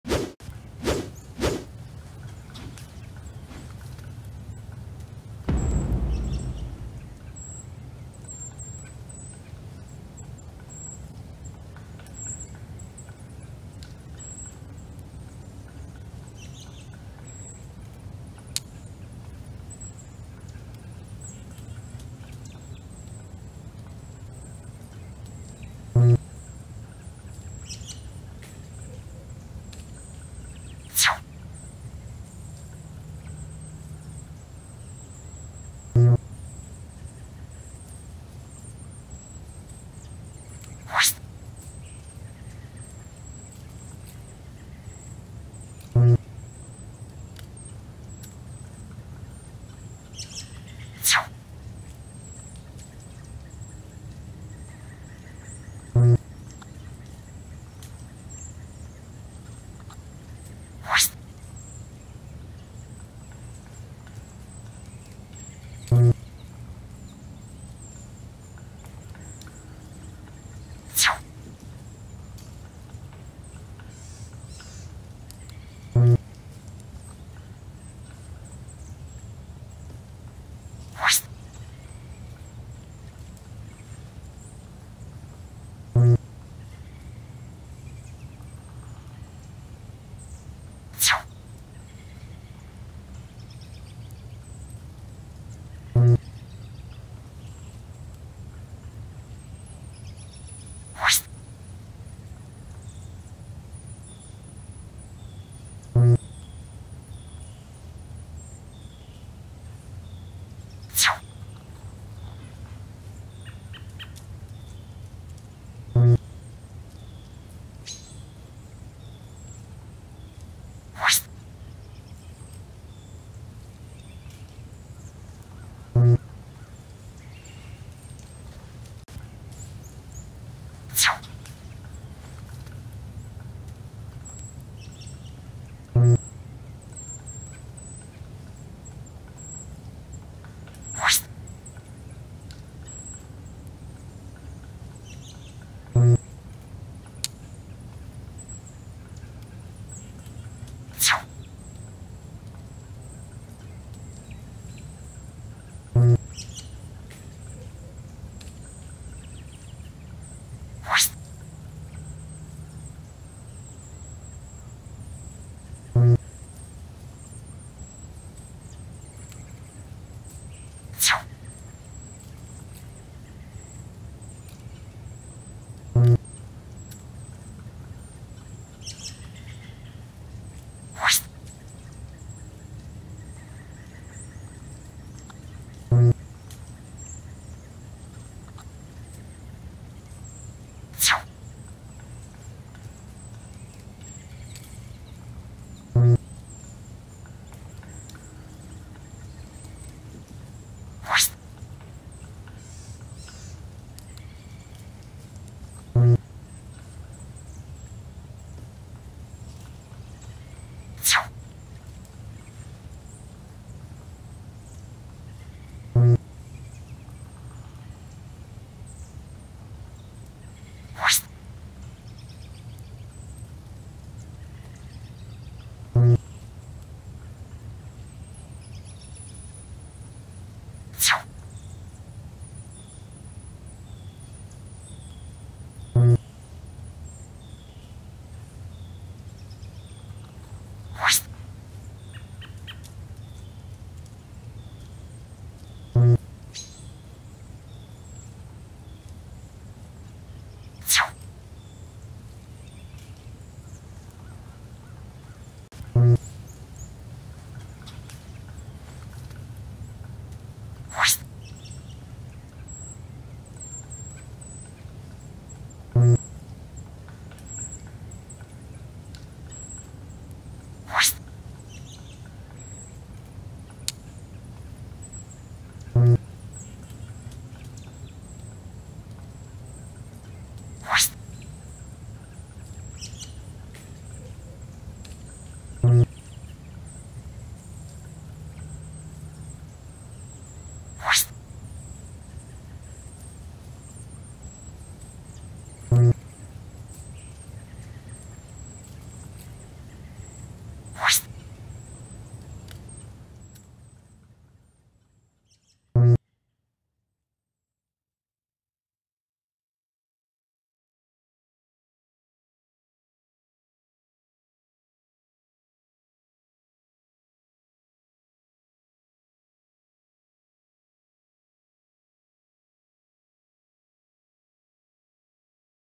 Viewer Request -Compare FirstLite Cipher VS Mossy Oak Bottomland with simulated deer vision.
Compare FirstLite Cipher VS Mossy Oak Bottomland on 14 Backgrounds with Simulated Deer and Human Vision.